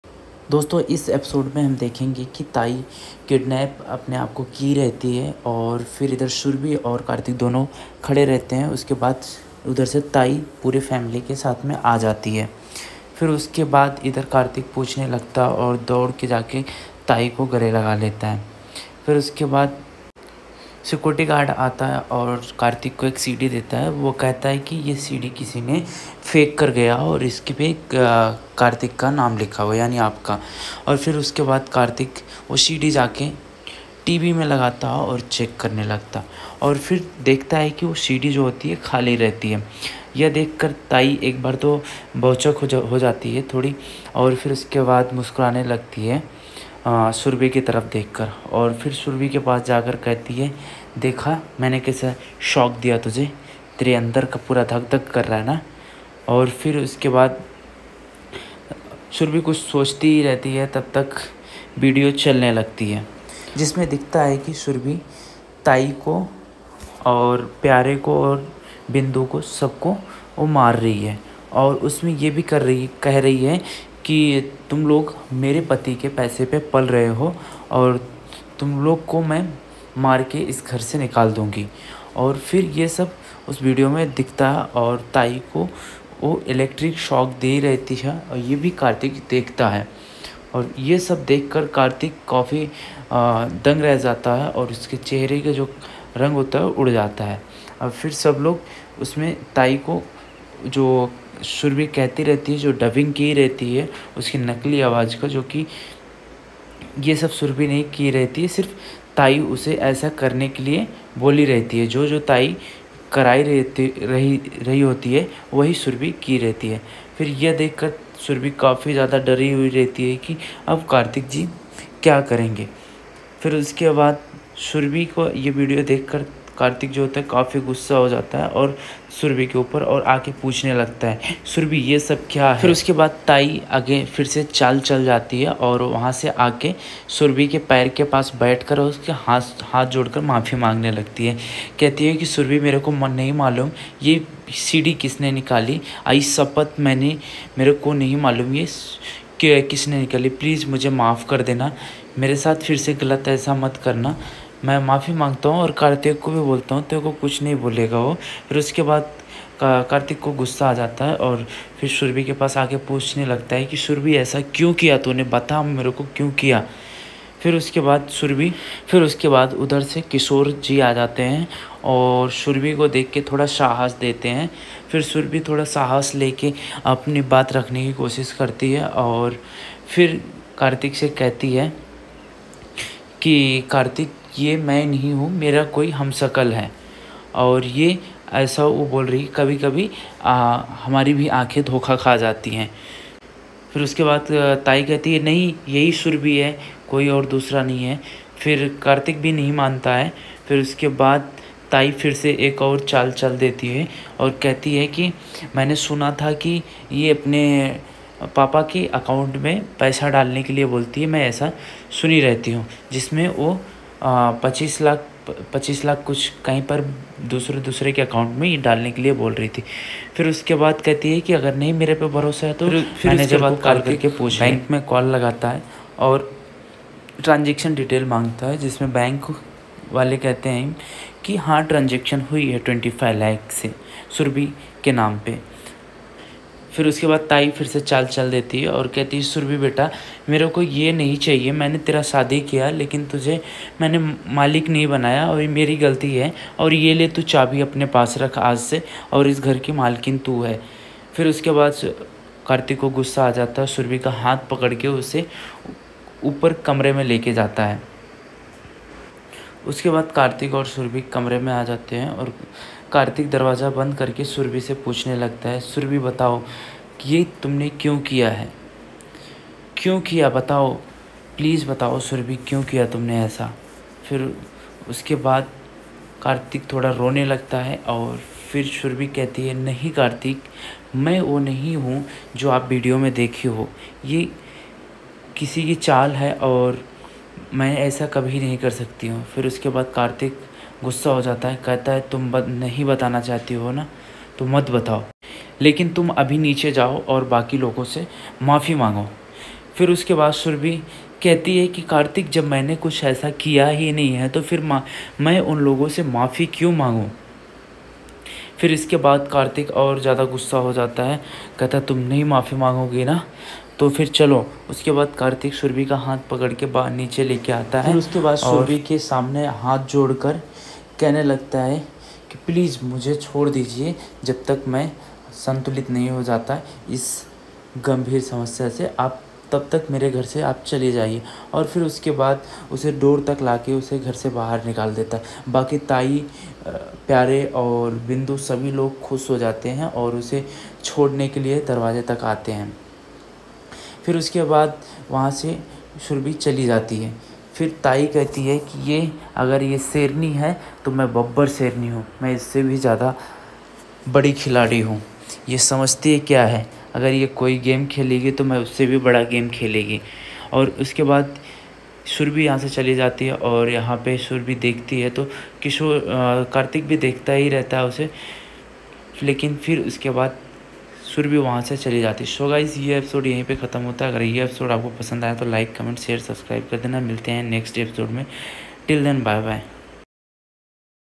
Yeh vaada raha 11 October कार्तिक ने सुरभि को निकाला घर से बाहर।
About Searchs.

This Video Images Credit By - Zee5 This Video is for Episode Explain Story News purposes only!!